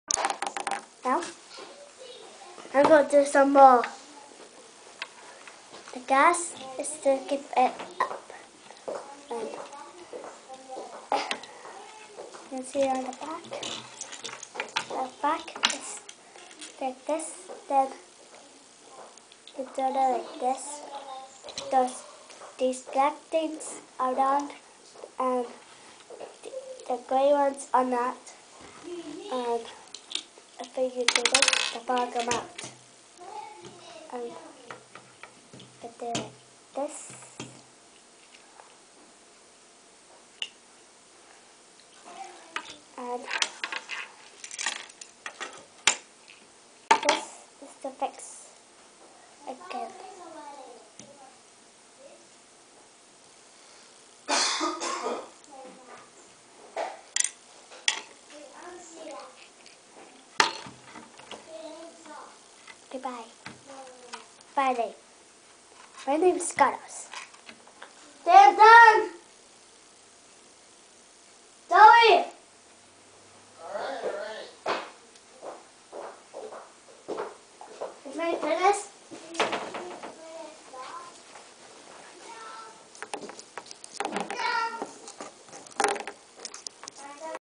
carlos lego power miner 2
child playing with legos